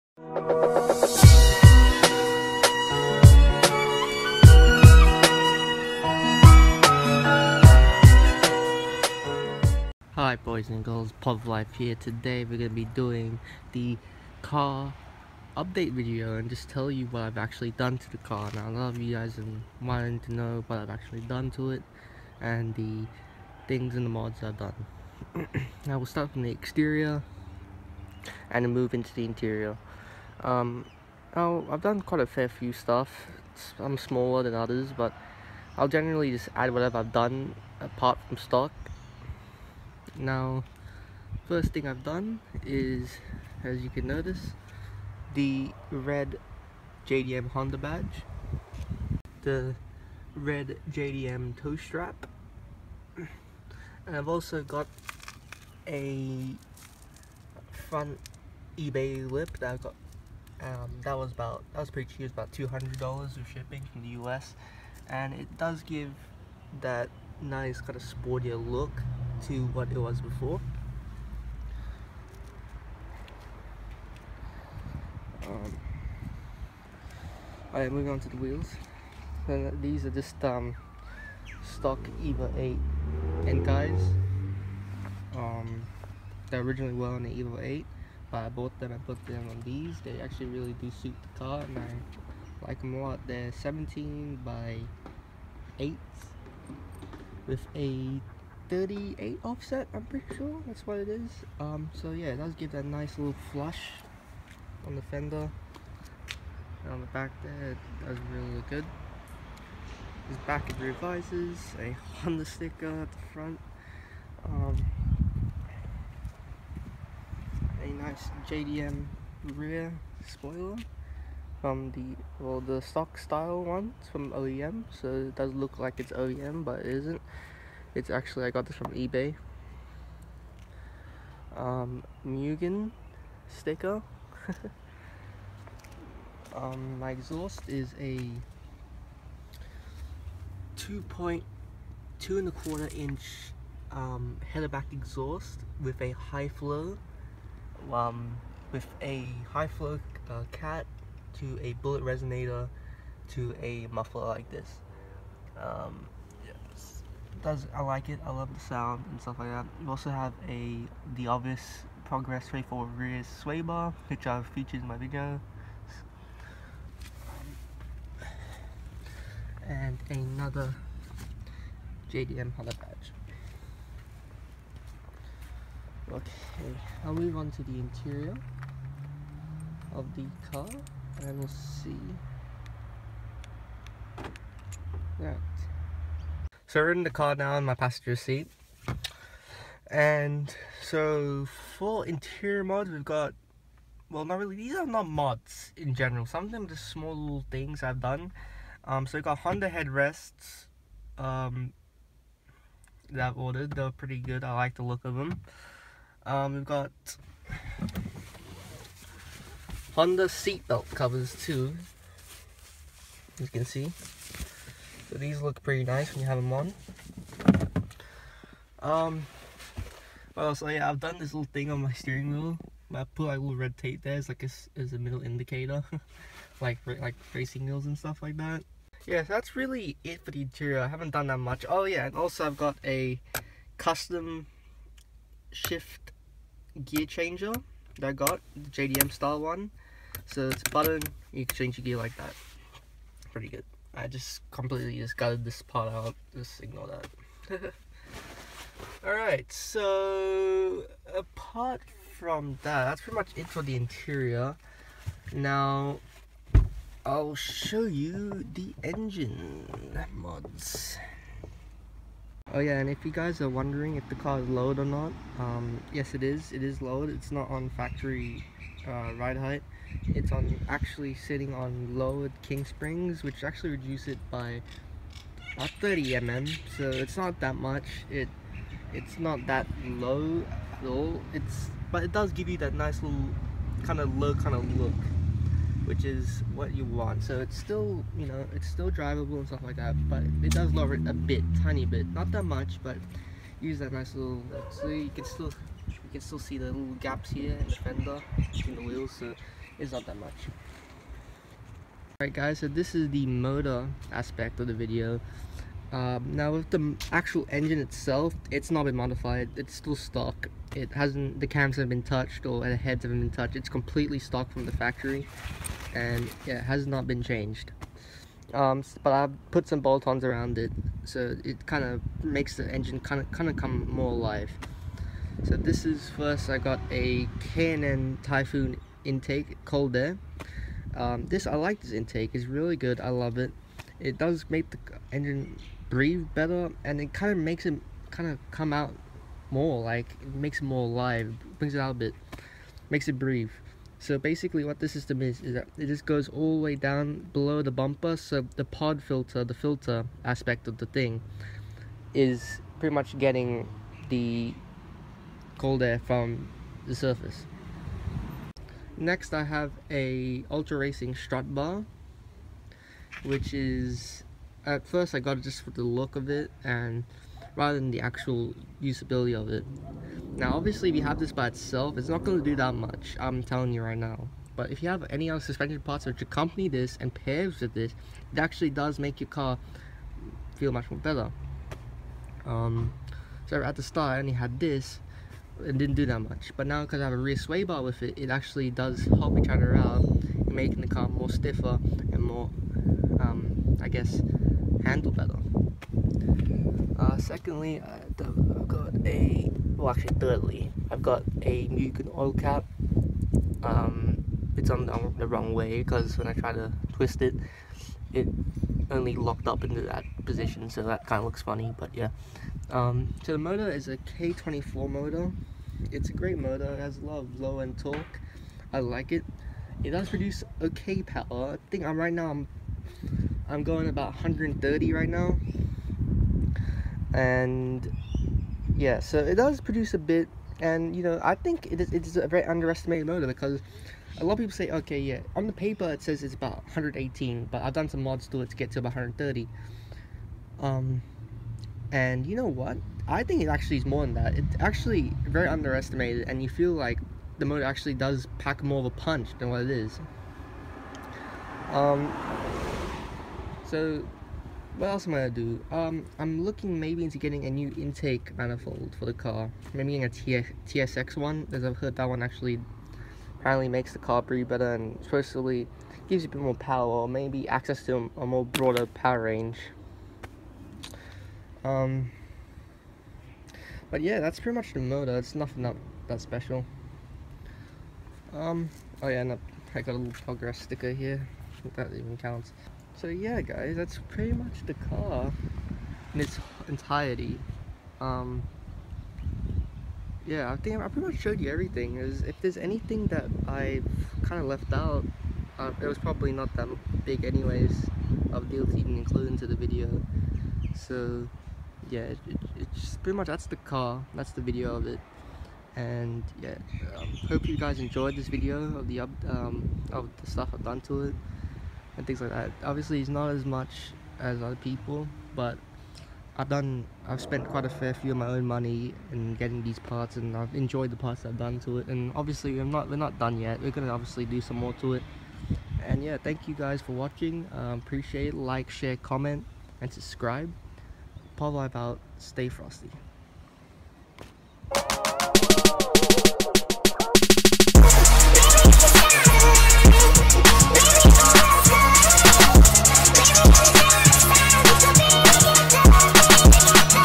2005 Honda Accord LX K24 Update Video
Driving Around In My Automobile, Capturing Everything which Amuses me in Australia's Largest City.

POVLife Here, just showing my 2005 Honda Accord and what I've done with it so far at this point of time, hope you guys enjoy.

All rights of the video are mine. Any reproduction for media/news/YouTube will not be allowed without prior written permission.